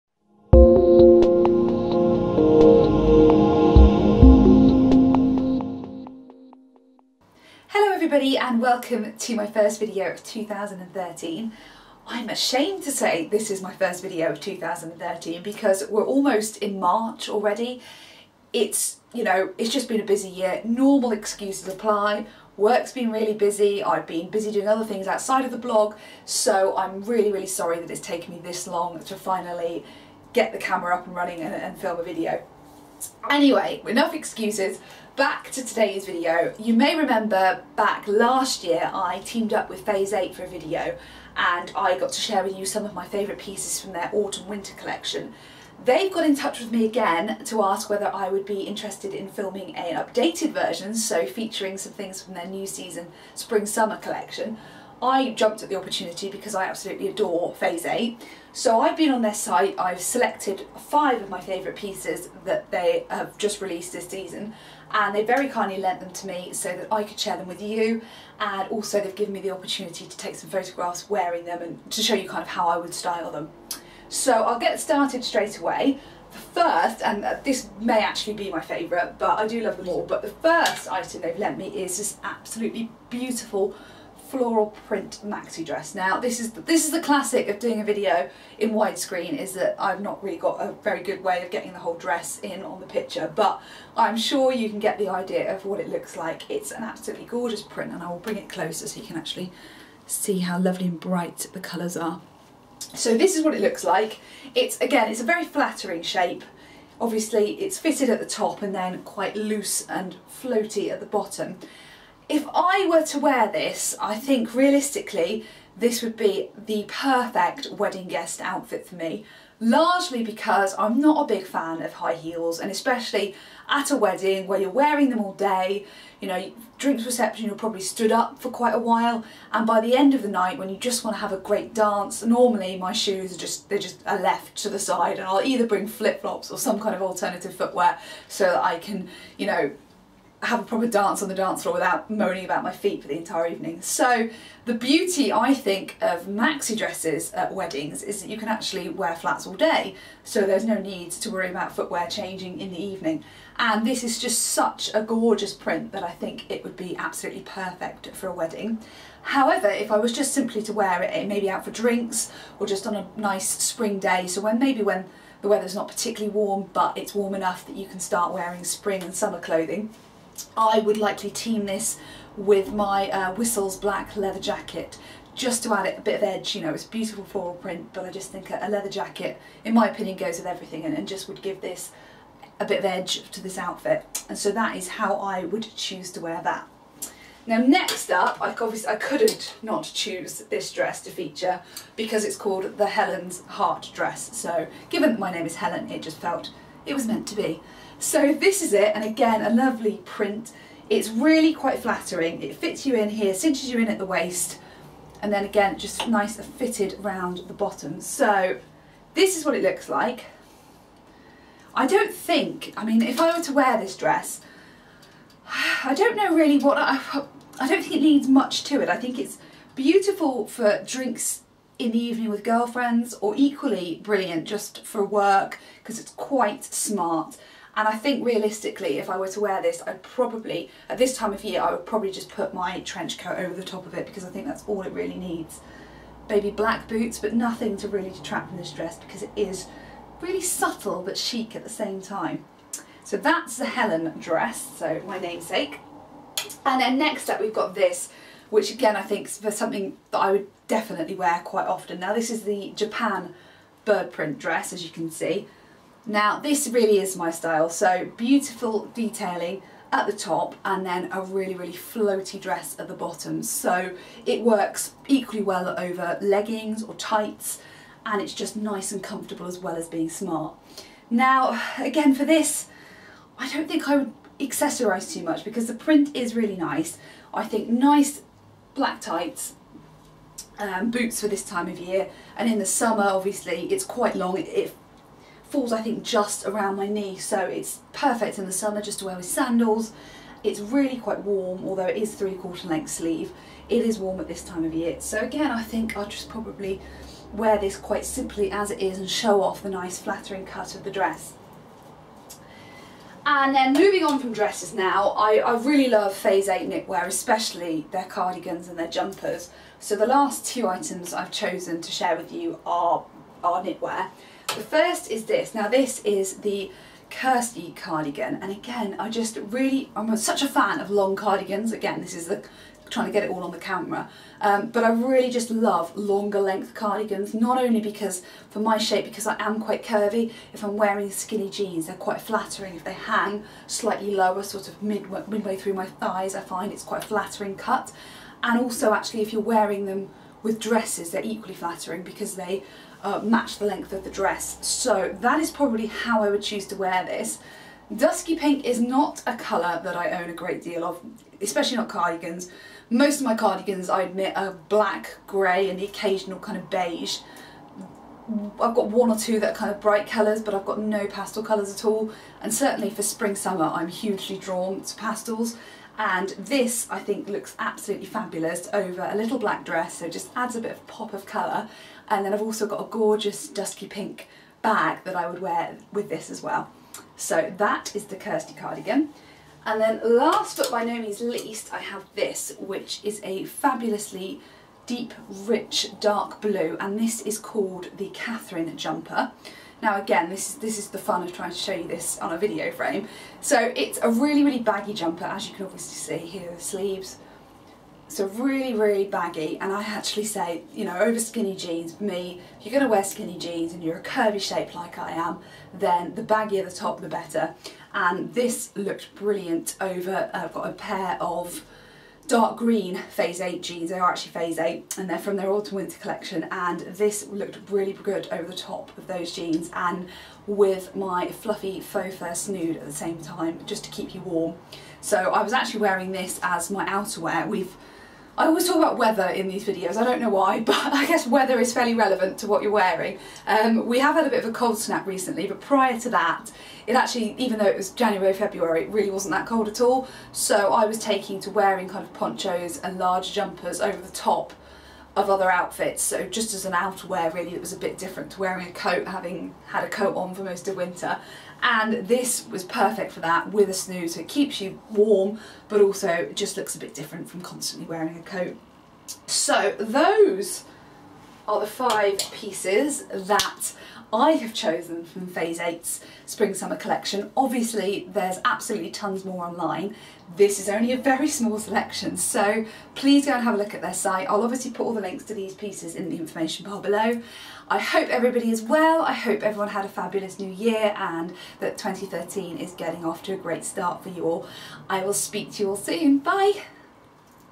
Phase Eight SS13 Collection
I selected a few of my favourite pieces from Phase Eight's SS13 Collection to share with you.

1. Peony Print Maxi Dress
2. Helen Hearts Print Dress
3. Japan Bird Print Dress